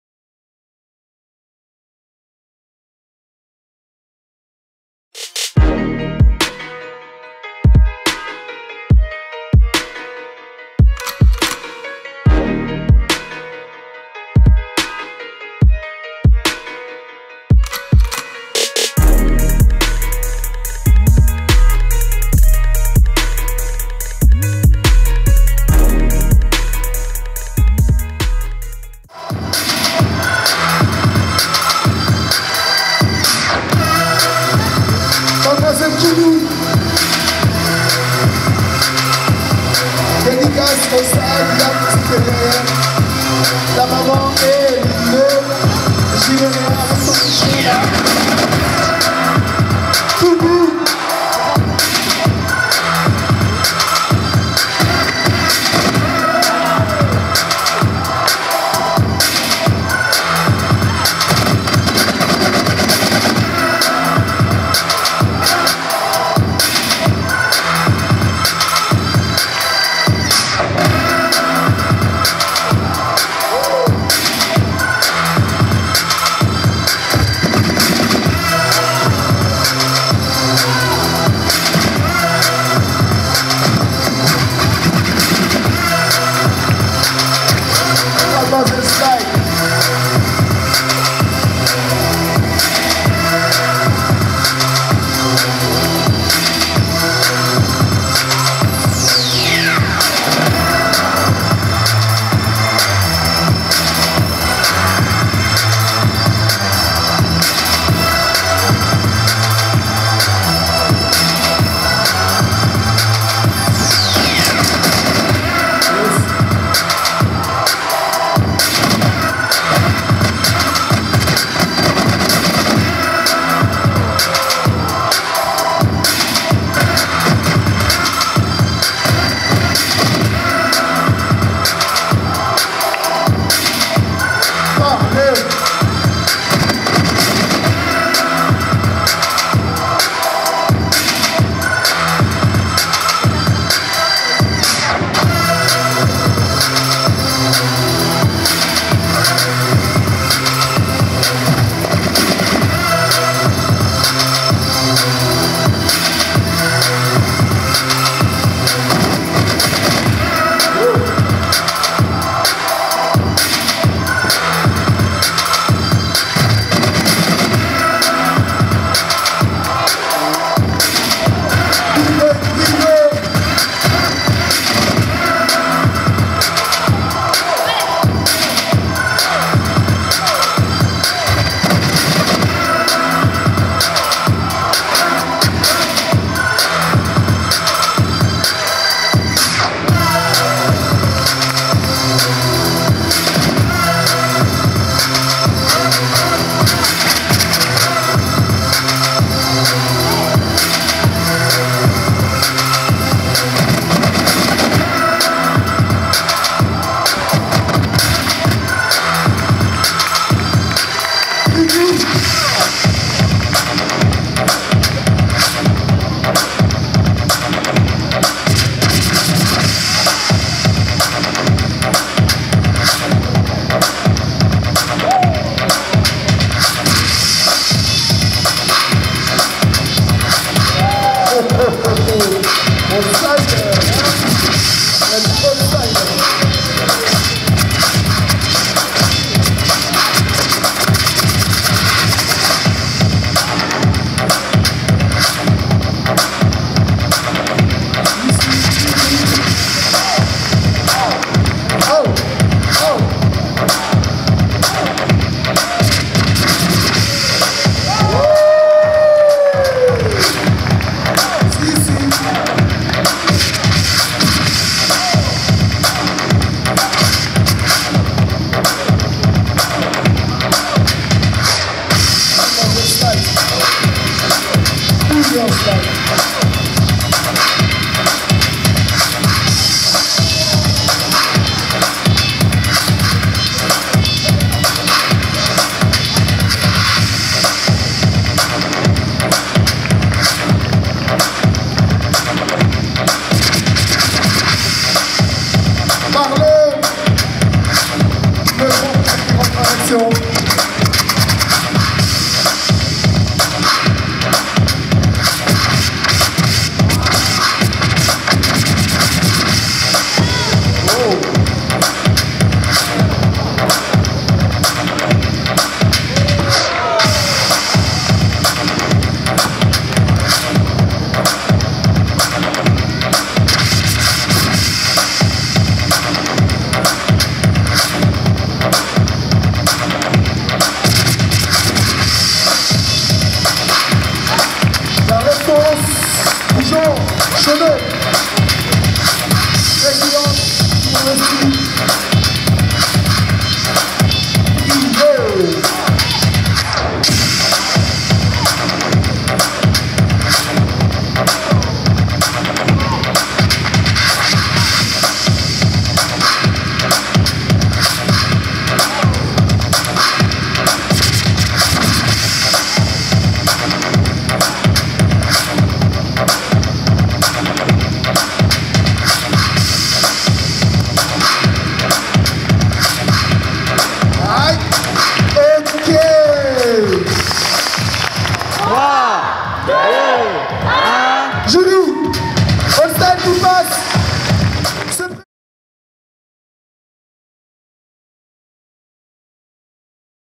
Quart de finale ► Battle Elle & Lui II ◄ break // Kimie & Marlone VS Bgirl Slidz & Bboy Brio
Organisation Cherihane Hdmi

Réalisation // Lebonbattle
Diffusion // Lebonbattle // BBoy World

avec HDMI Team , Faouzy Hdmi-Phaset , Julien Fio 😂👍🌎🌏🌍

Speaker / Faouzy Hdmi-Phaset

dj / Dj Nessone

Jury / Sarah Bee , Cedric CedRock , Bob